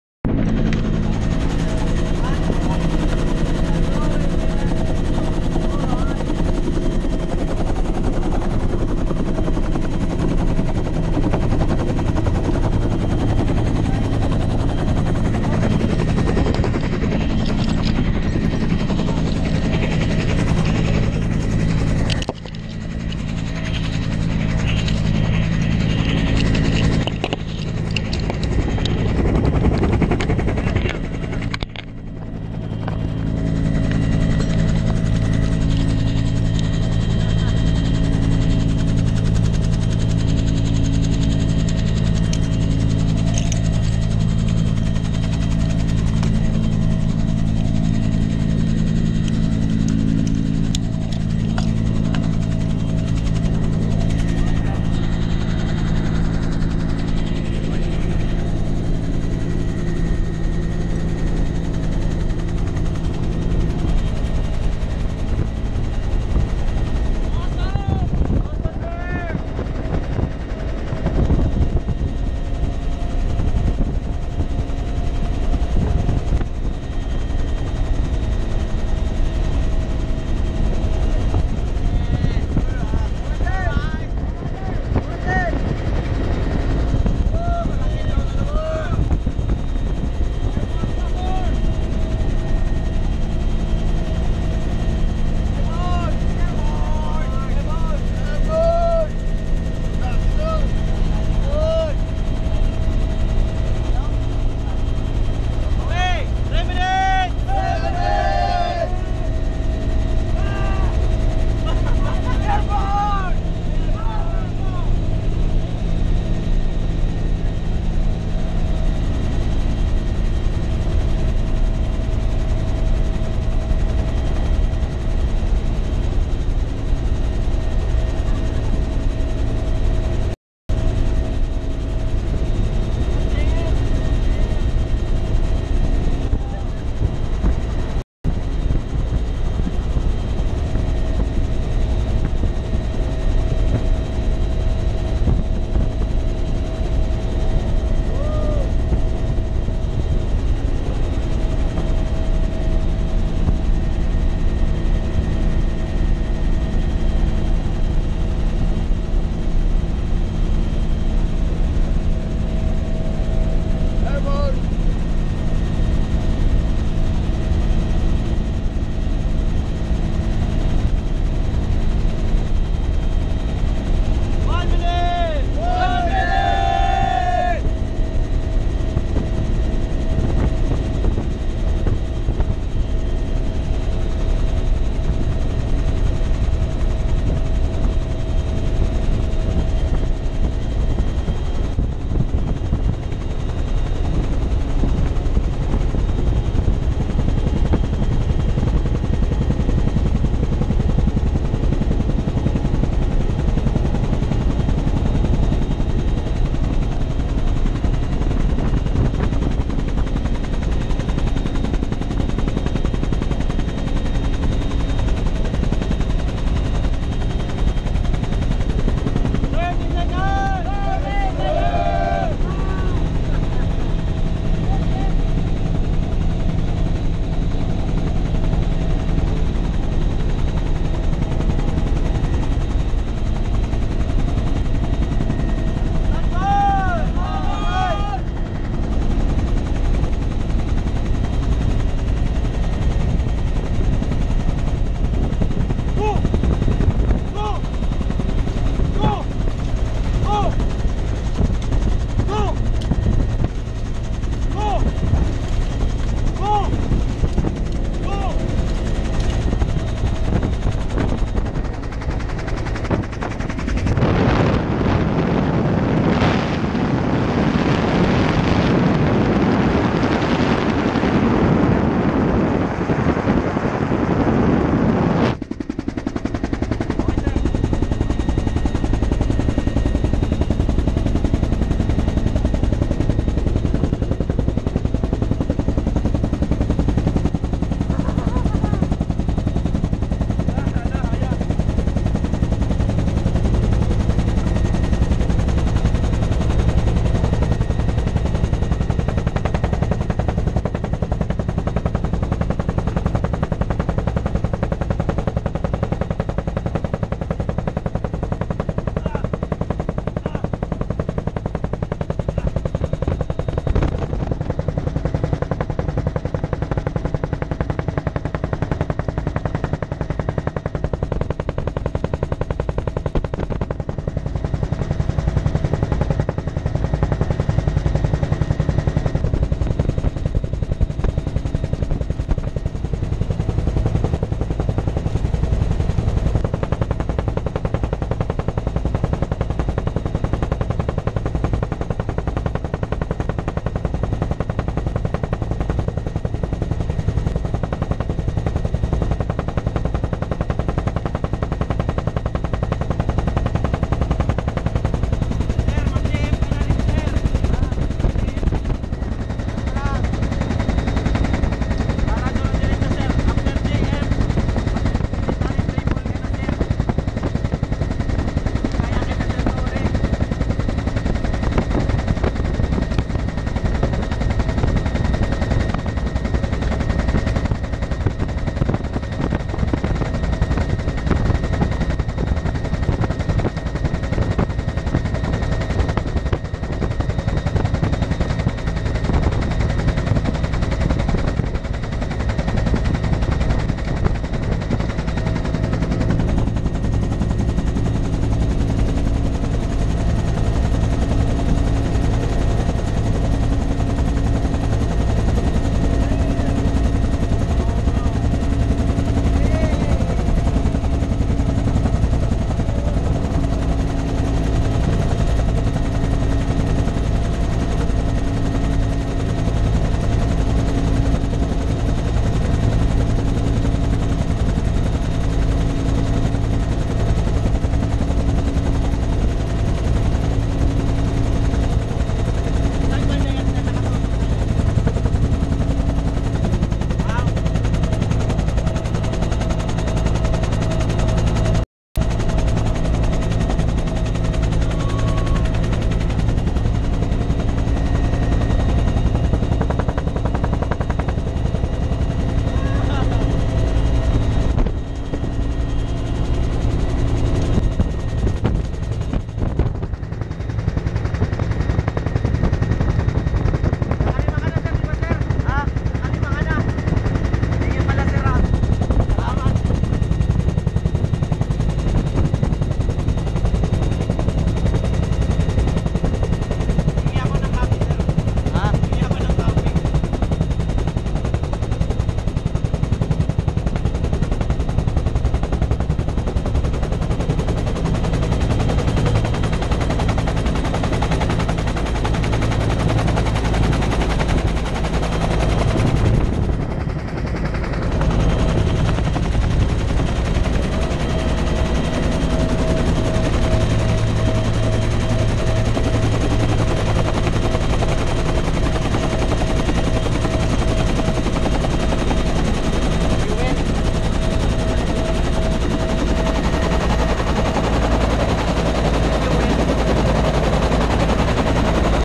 MILVLOG # 13 - Being a Marine Jump Master (Phil Marines Basic Airborne Course Class - 44)
Play on HD for best quality.
Keep in touch with me and Follow me Guys on my

This video was captured by one of my students during the Marine Basic Airborne Course Class - 44 year 2016. I, also is a student of the Marine Jump Master Course Class - 40 and I am proud as well as confident to share this video of me performing what I learned in Jump Master Course.